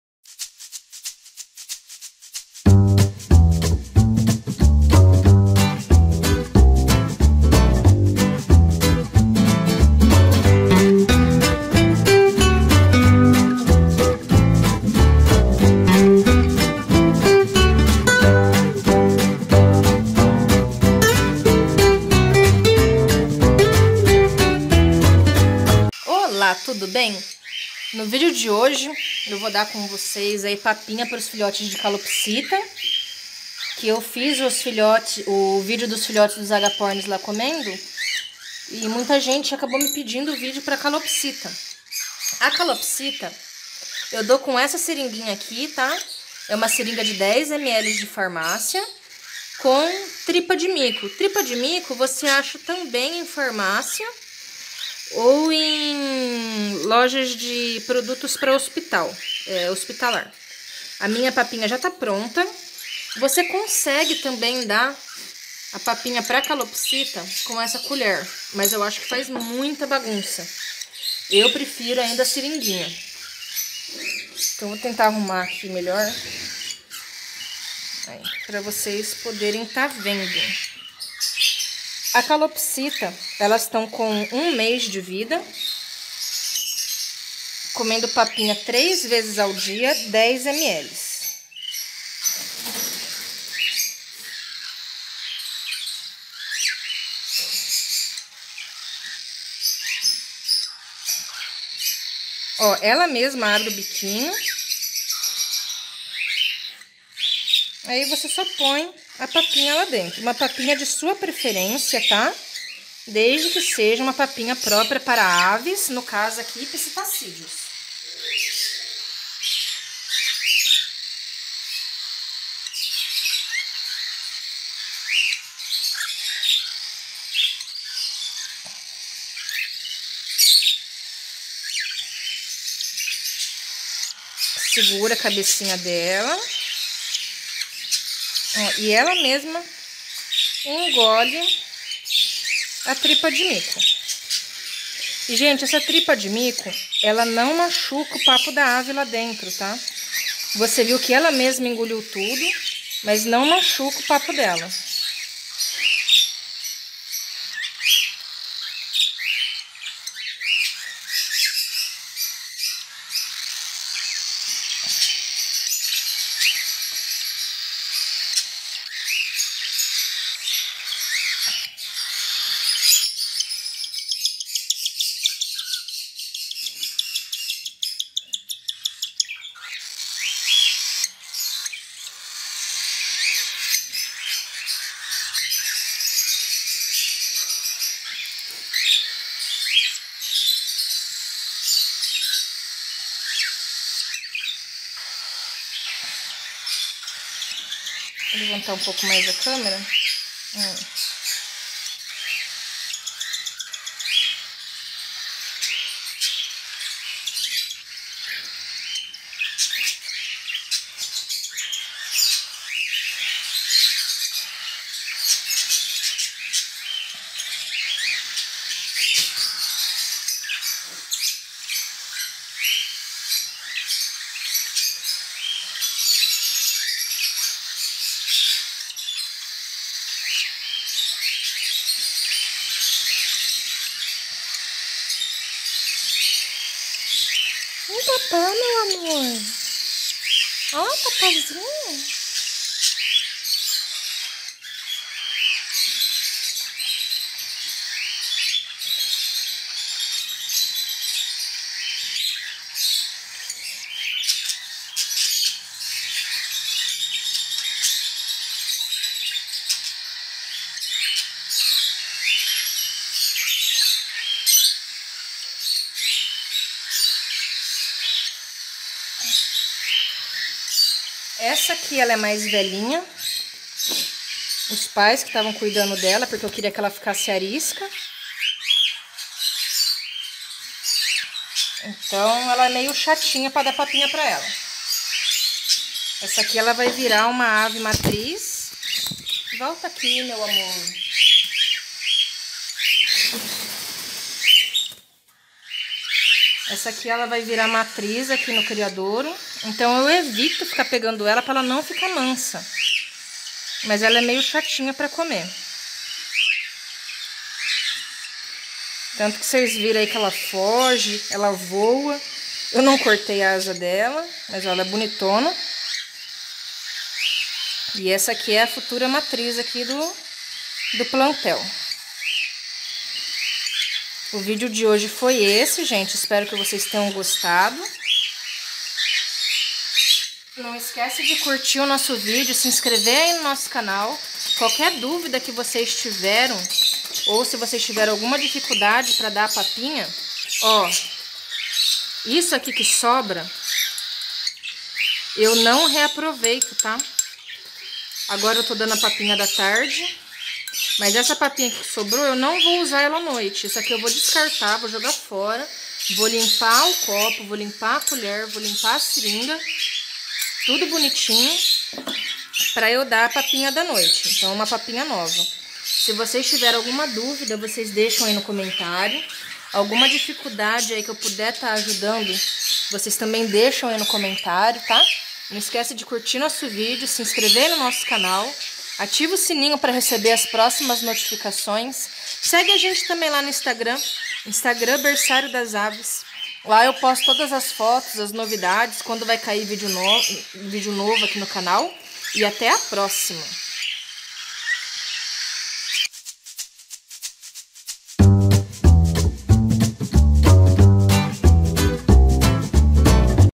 Dando Papinha para Filhotes de Calopsita
No vídeo de hoje mostro para você como alimentar os filhotes de calopsita usando a seringa.